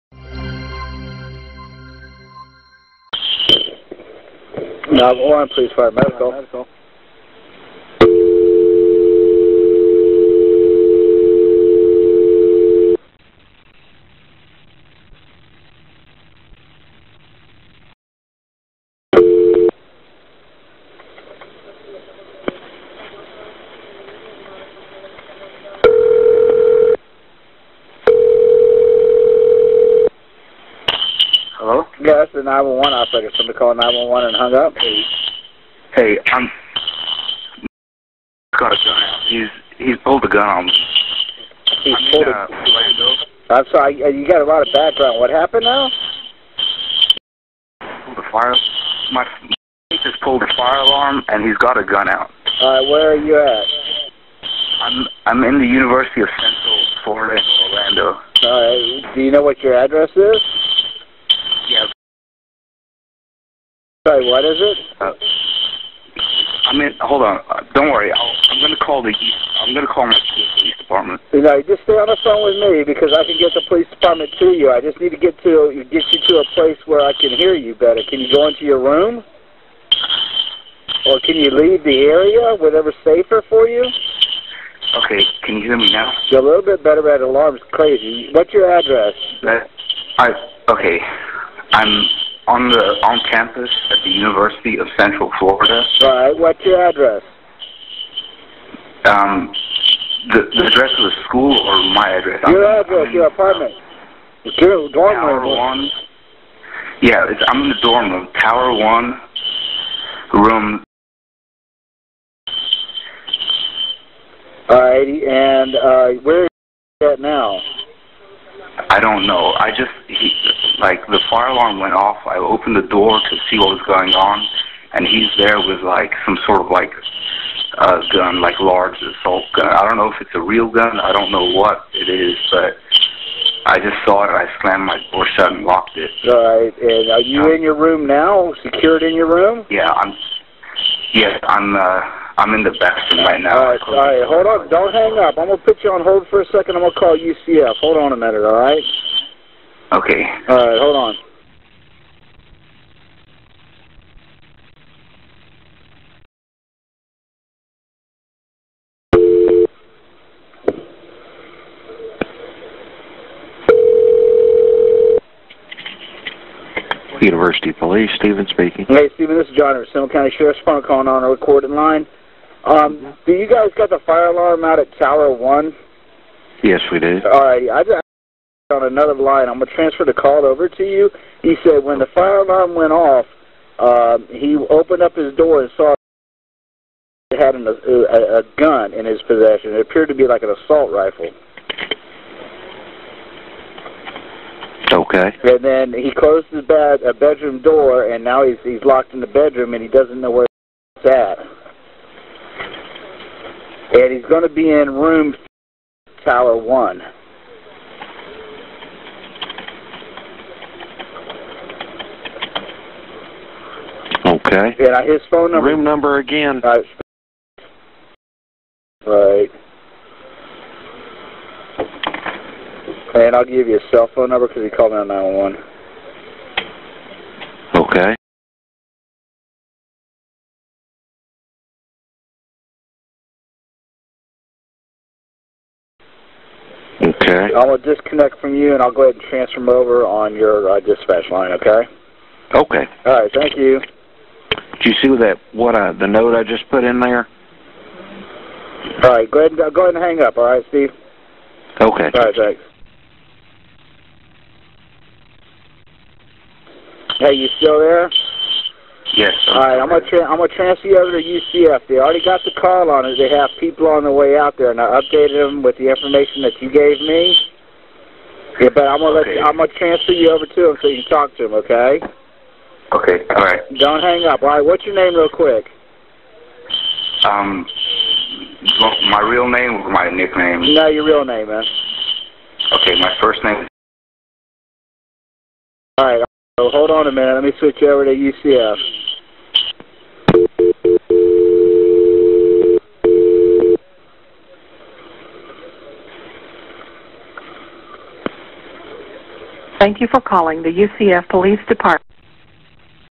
911 call: Roommate of accused UCF plotter
The roommate of accused UCF attack plotter James Oliver Seevakumaran called 911 after seeing Seevakumaran with a gun in their dorm room.